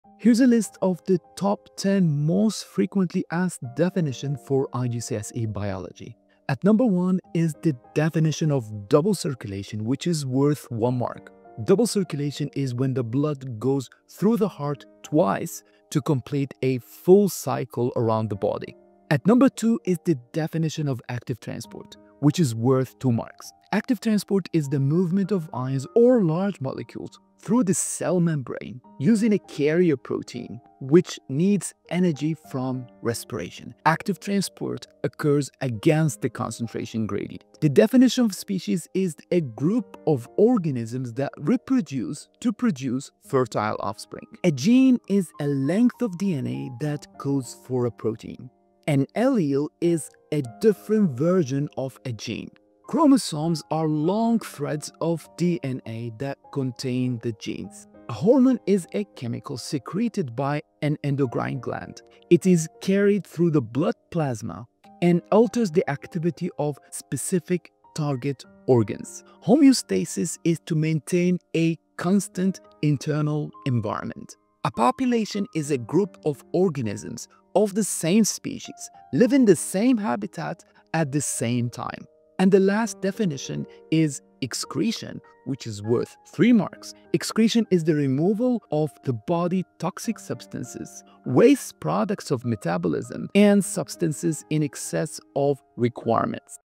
The top 10 most asked IGCSE Biology definitions for the 2026 exam
This is a short segment from the IGCSE Biology Course for 2026

 • 📚 Top 10 IGCSE Biology Definitions for the 2026 exams
 • 🥇 No. 1: Double Circulation (1 Mark)
 ▪ Definition: Blood goes through the heart twice to complete a full cycle around the body.
 • 🥈 No. 2: Active Transport (2 Marks)
 ▪ Definition: Movement of ions or large molecules through the cell membrane using a carrier protein.
 ▪ Requires energy from respiration.
 ▪ Occurs against the concentration gradient.
 • 🧬 Species
 ▪ Definition: A group of organisms that reproduce to produce fertile offspring.
 • 🧬 Gene
 ▪ Definition: A length of DNA that codes for a protein.
 • 🧬 Allele
 ▪ Definition: A different version of a gene.
 • 🧬 Chromosomes
 ▪ Definition: Long threads of DNA that contain the genes.
 • 💉 Hormone
 ▪ Definition: A chemical secreted by an endocrine gland.
 ▪ Carried through the blood plasma.
 ▪ Alters the activity of specific target organs.
 • ⚖️ Homeostasis
 ▪ Definition: Maintaining a constant internal environment.
 • 🌍 Population
 ▪ Definition: A group of organisms of the same species living in the same habitat at the same time.
 • 🚽 Excretion (3 Marks)
 ▪ Definition: Removal of toxic substances, waste products of metabolism, and substances in excess of requirements from the body.